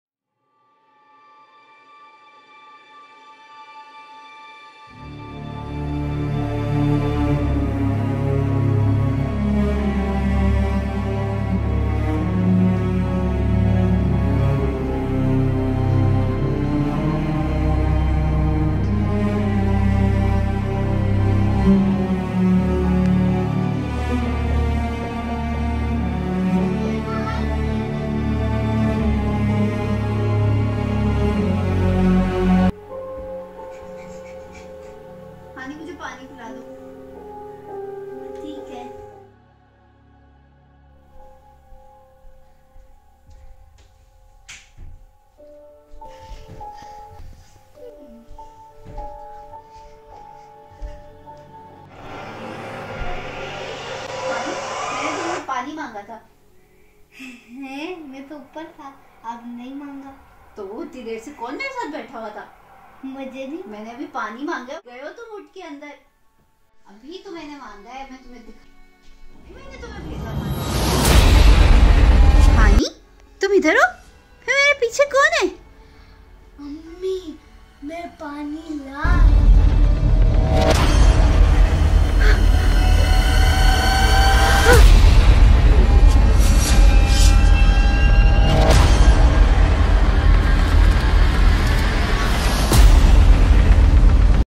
The mysterious Son
A mother asks her son for water, but a spine-chilling twist awaits as he heads to the kitchen